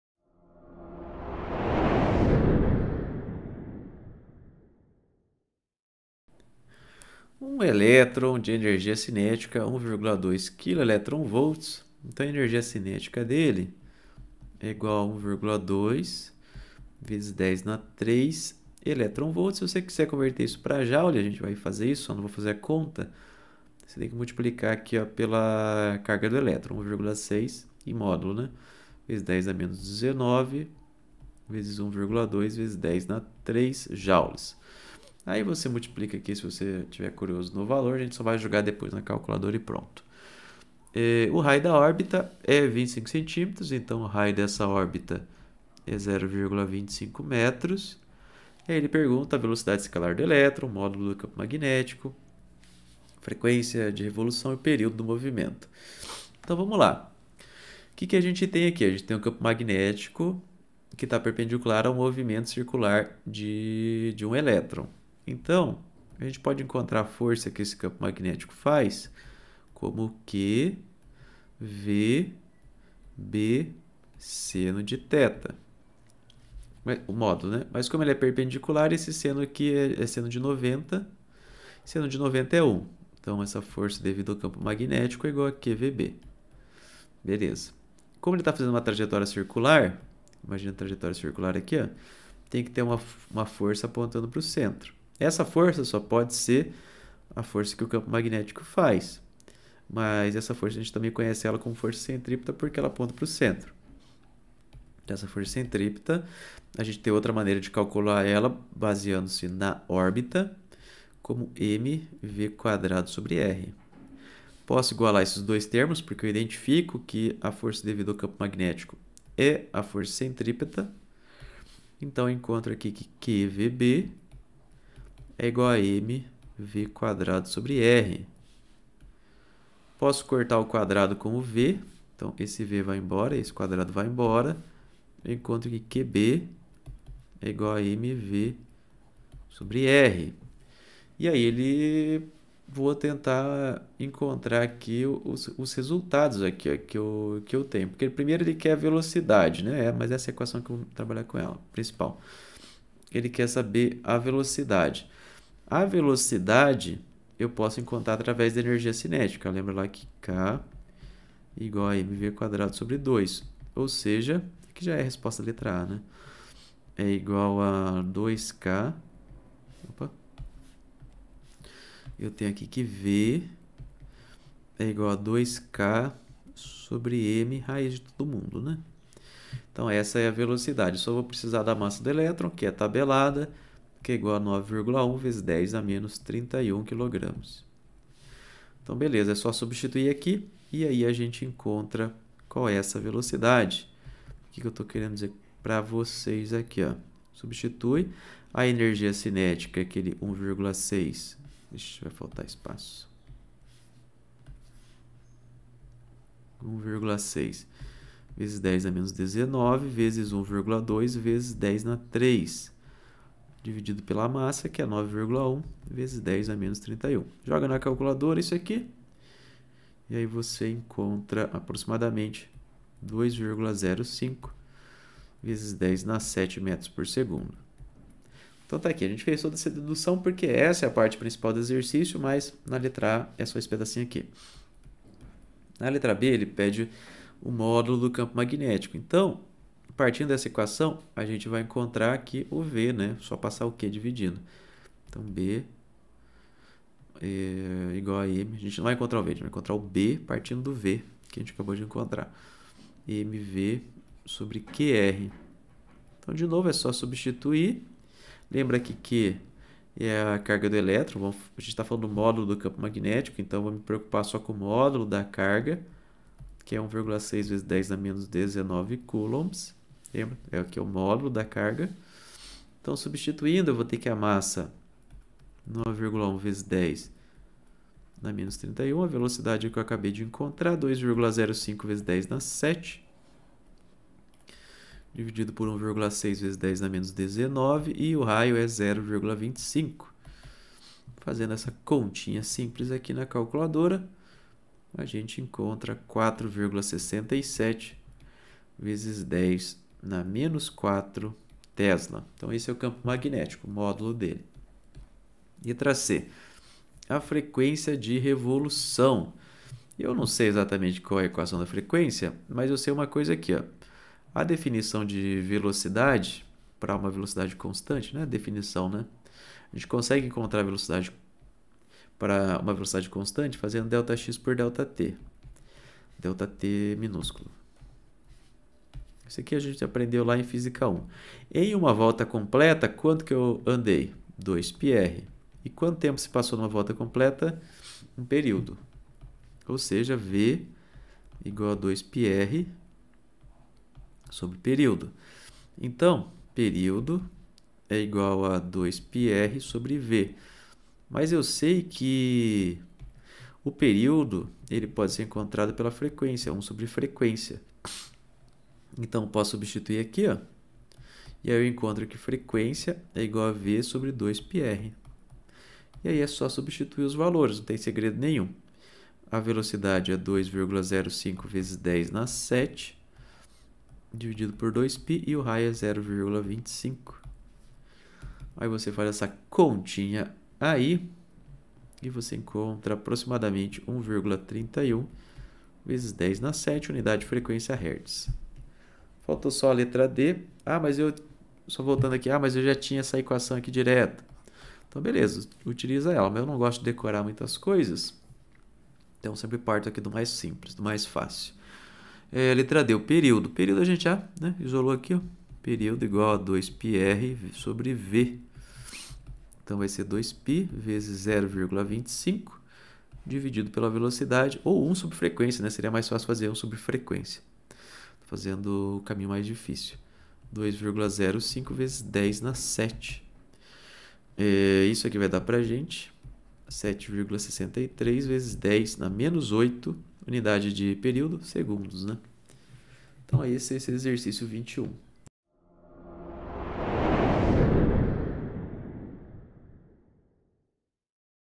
(21-28) Exercício Resolvido - Halliday (Exercício 21 Capítulo 28)
Um elétron de energia cinética 1,20 keV descreve uma trajetória circular em um plano perpendicular a um campo magnético uniforme. O raio da órbita é 25,0 cm. Determine (a) a velocidade escalar do elétron, (b) o módulo do campo magnético, (c) a frequência de revolução e (d) o período do movimento.